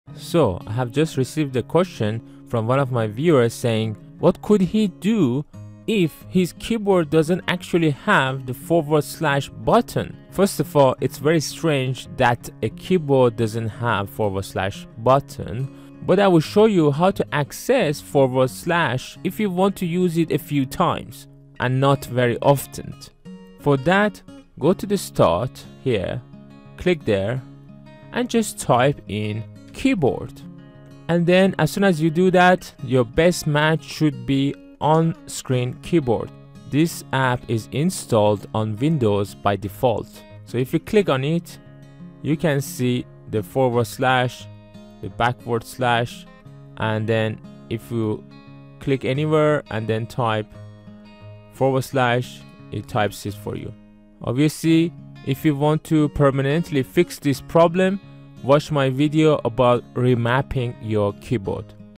How to type forward slash (/) with no forward slash key
In this quick video, I'll show you how to insert forward slash on Windows 10. I have made a lot of other videos about how to find the forward slash or backward slash on the keyboard. However, if you rarely use forward slash, then you could use the on-screen keyboard app on Windows 10 to access all the keys.